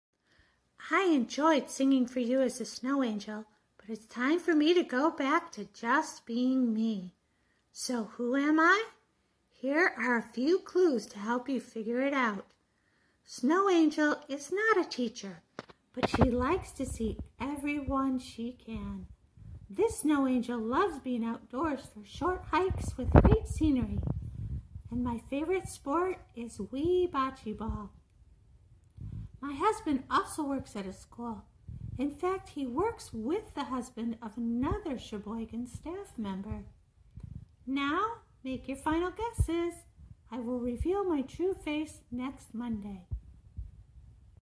Cheboygan Masked Singer - Snow Angel Clues
Our first eliminated contestant is The Snow Angel. Her true identity will be revealed on Monday, December 7. In the meantime, here are a few clues to help you figure it out.